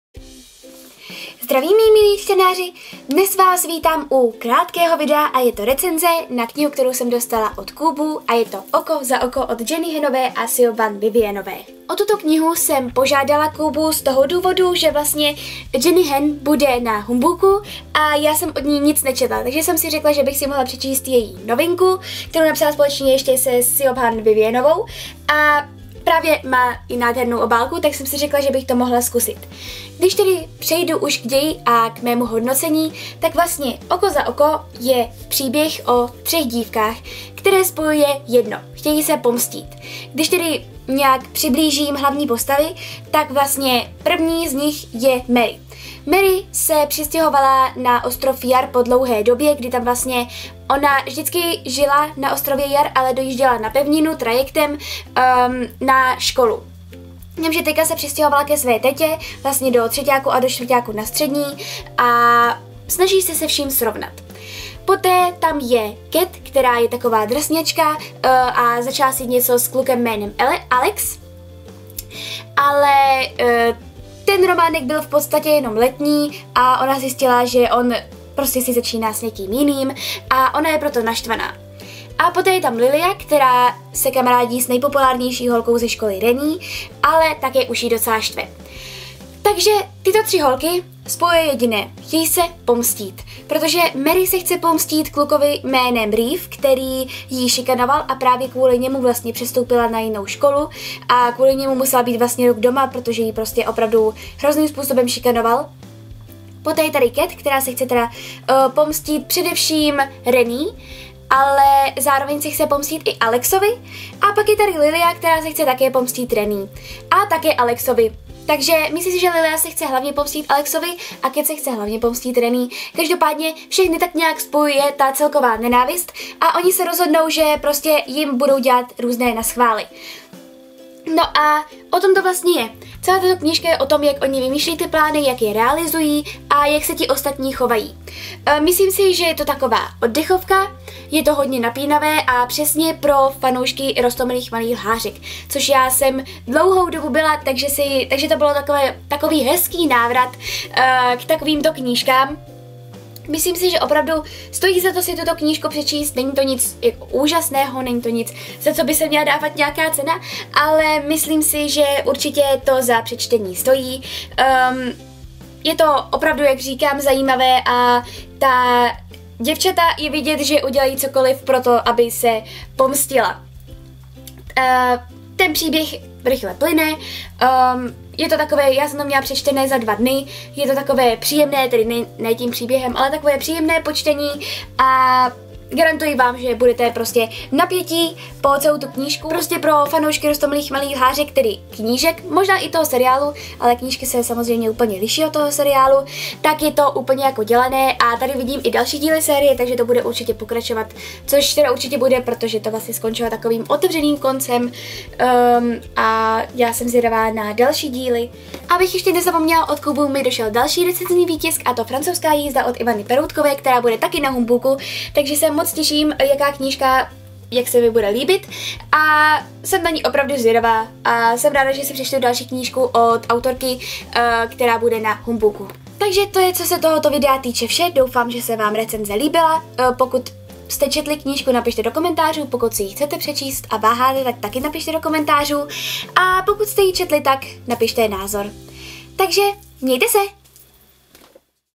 Klářiny knihy -Minirecenze knihy Oko za oko
Zdravím, mí milí čtenáři!
Dnešní video je minirecenzí knihy Oko za oko od Jenny Hanové a Siobhan Vivianové. Knihu byste si měli přečíst už jen z toho důvodu, že Jenny bude i na Humbooku!